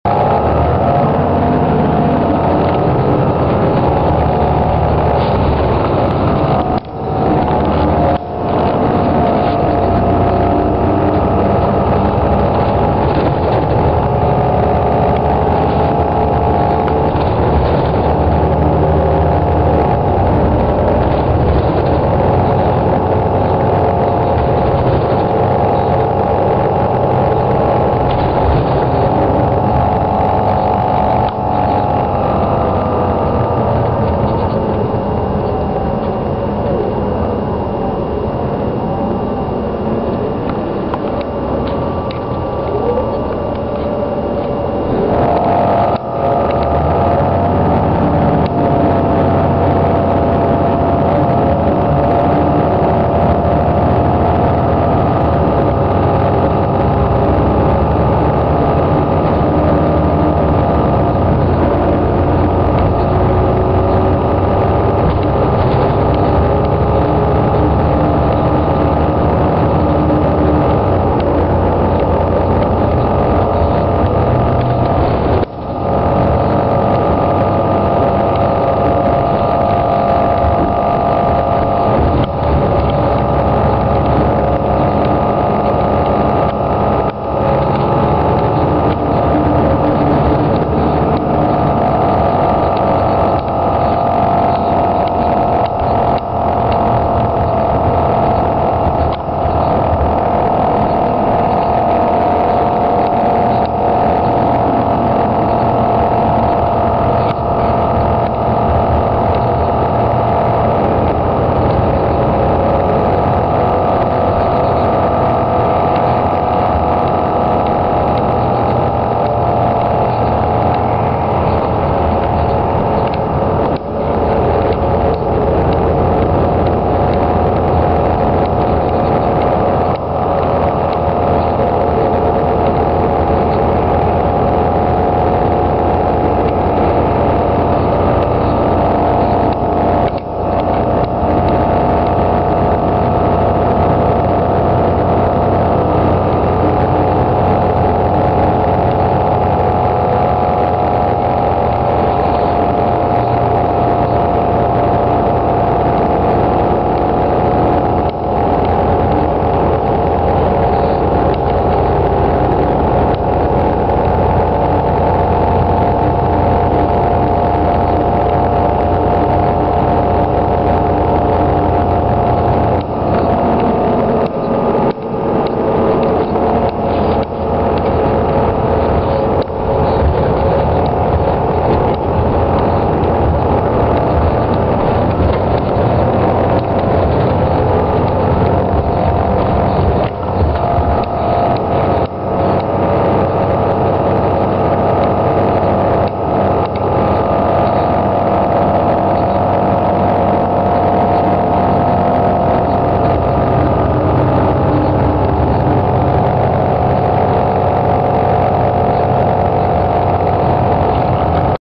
GRTC Bus #1502
As dark as it may be, the ride on this CAT C-13 powered MCI D4505 is definately worth it. This is GRTC Bus #1502 on the Gaskins Express (route 29). This bus has a Allison B500R transmission.